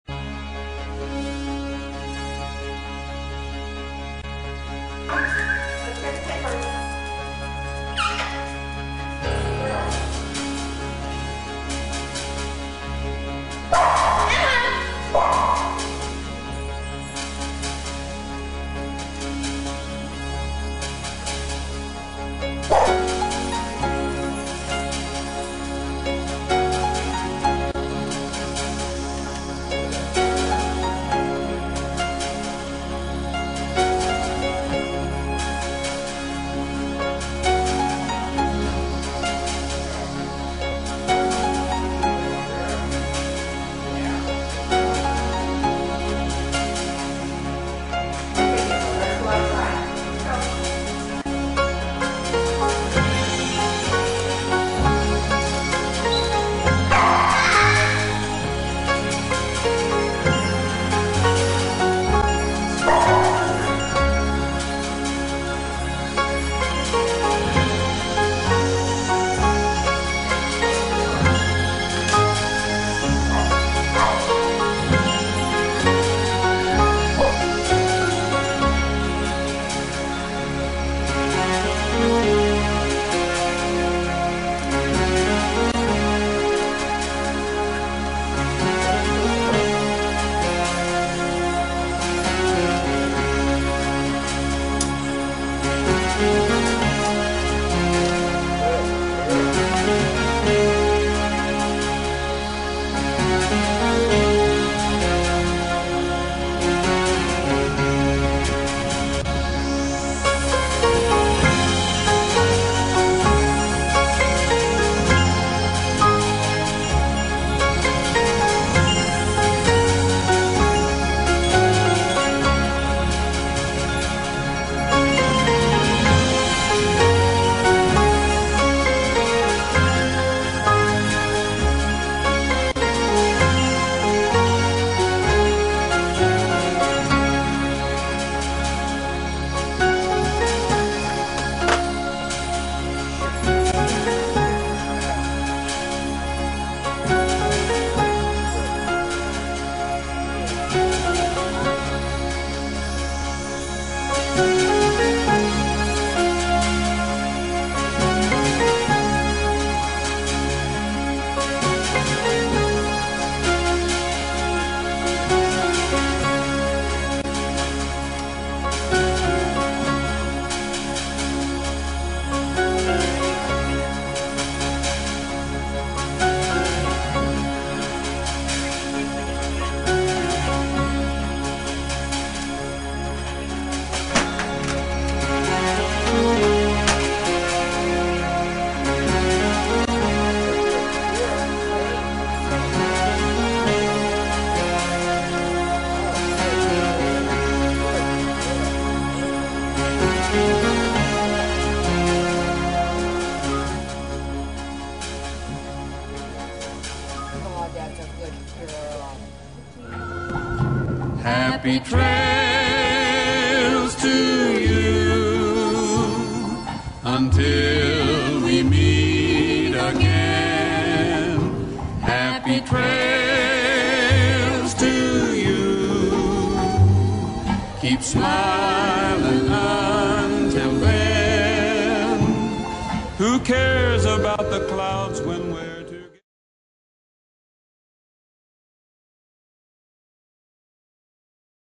Gizmo the Service Dog
Watch Gizmo perform her tasks as she works at home and during a trip to the store.